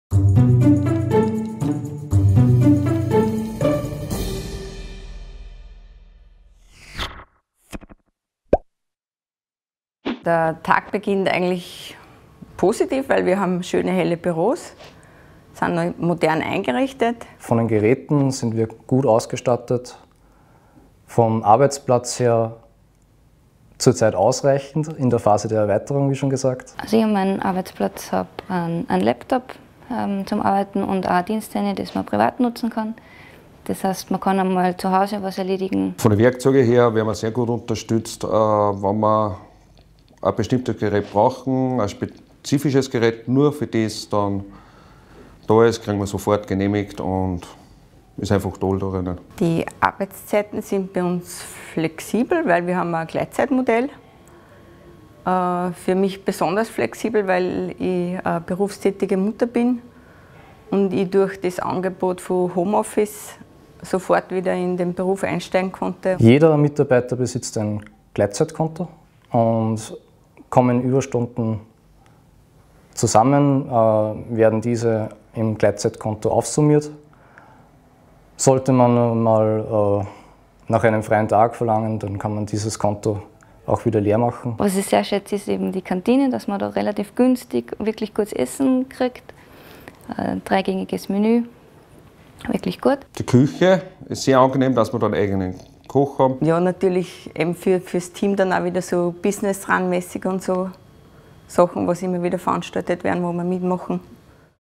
Welche Rahmenbedingungen bringt der Job mit sich? - VFI GmbH auf karriere.at
Der karriere.reporter von karriere.at bei VFI GmbH:

Interview mit:
Sylvia Lindlbauer Einkauf Packstoffe
Julia Maria Imböck Qualitätsmanagement
Roland Lettner  Instandhaltung
Kurt Petraschek  Labor/Qualitätssicherung

Der Tag beginnt bei Silvia sehr positiv in einem schönen, hellen, modern eingerichtetenBüro. Ein flexibles Gleitzeitmodell kommt ihr als Mutter sehr entgegen.
Kurts Arbeitsplatz ist trotz Mitarbeiterwachstums durchaus ausreichend. Überstunden können als Zeitausgleich konsumiert werden.
Julia Maria hat Laptop und Diensthandy und kann diese auch privat nutzen. Die Kantine mit eigenem Koch findet Julia Maria ganz besonders praktisch.
Roland fühlt sich sehr gut unterstützt, wenn Geräte neu angeschafft werden müssen